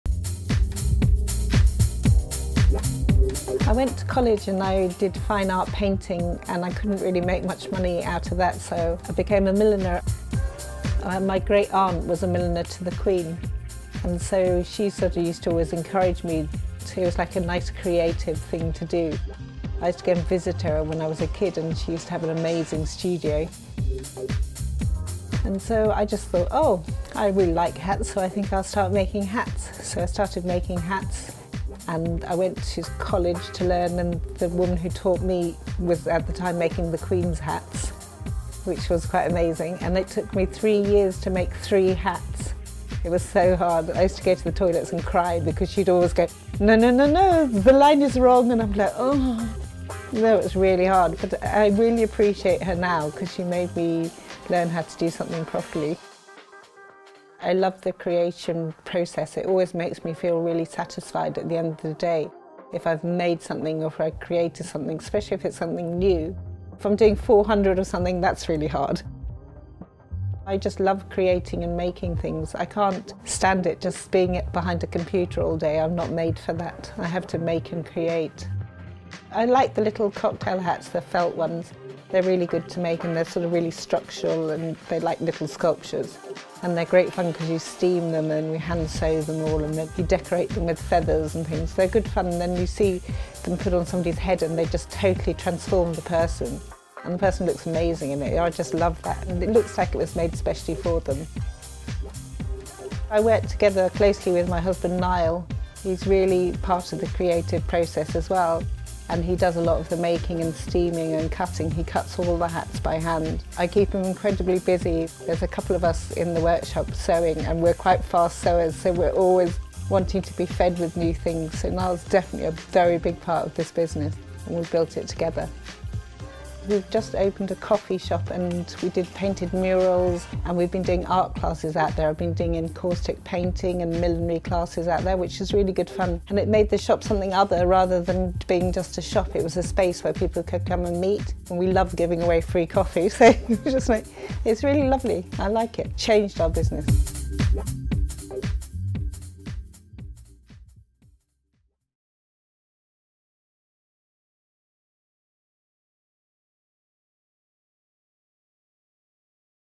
Kathleen McAuliffe Dingle
This lovely short feature about Kathleen was filmed and edited by Bláthnaid O'Toole